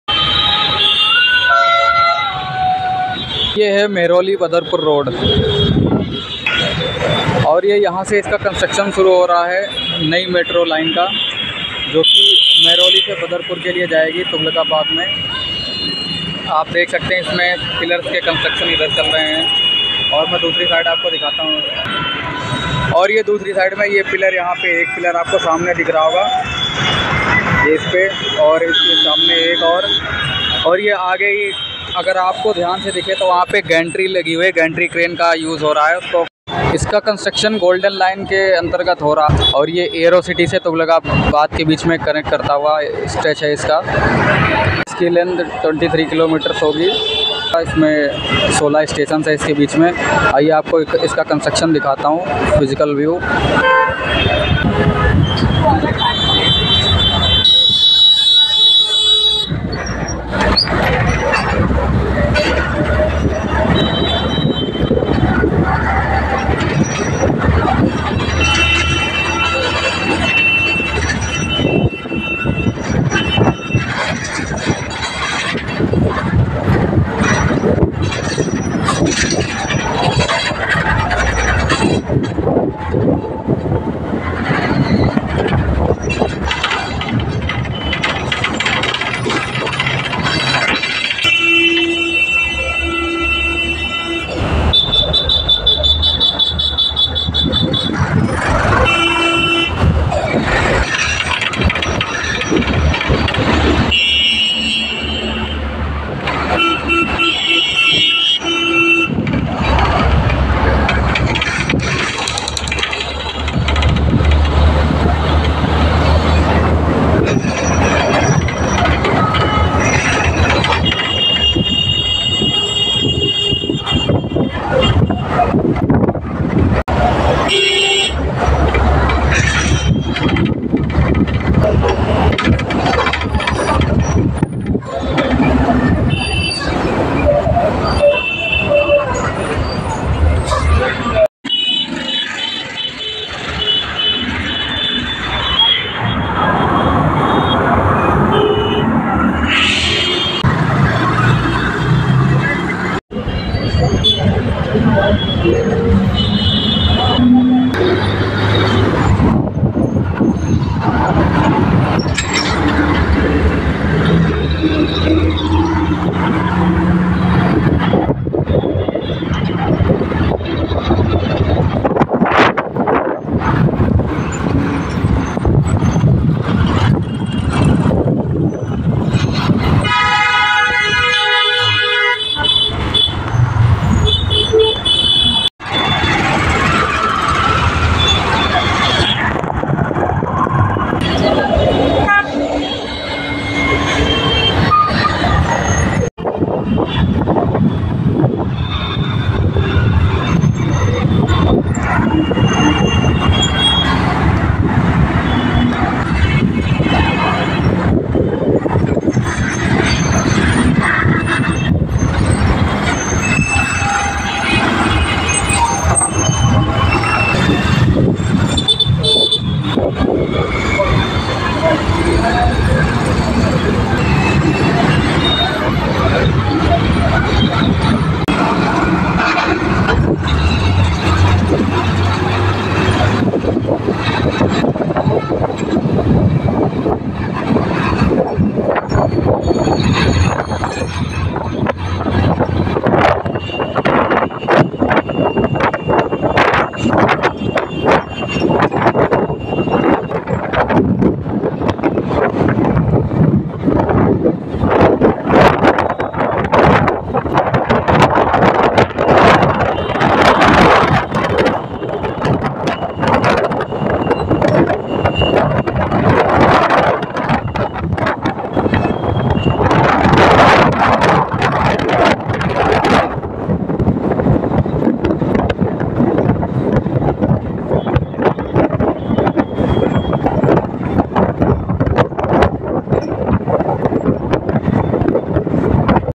Delhi metro phase 4 Construcion Golden line from Delhi Aerocity to Tughlakabad Badarpur
Title: Exploring Delhi Metro Phase 4: The Golden Line Construction

Description: Join us on a captivating journey through the construction of Delhi Metro's Phase 4 Golden Line! Discover the ambitious expansion plans of India's capital city as it gears up to connect even more neighborhoods with seamless metro transit.

In this video, we delve into the intricacies of the Phase 4 project, focusing on the Golden Line construction, which promises to revolutionize Delhi's public transportation landscape. From groundbreaking ceremonies to the laying of tracks, witness the progress and challenges encountered along the way.

Learn about the strategic route planning, advanced engineering techniques, and innovative technologies employed to ensure the timely completion of this transformative metro line. Explore the impact of the Golden Line on easing congestion, reducing commute times, and enhancing urban connectivity for millions of Delhiites.

Whether you're a metro enthusiast, urban planner, or simply curious about infrastructure development, this video offers a fascinating glimpse into the future of transportation in Delhi. Join us as we embark on this exciting journey towards a more connected and sustainable cityscape with Delhi Metro Phase 4's Golden Line construction!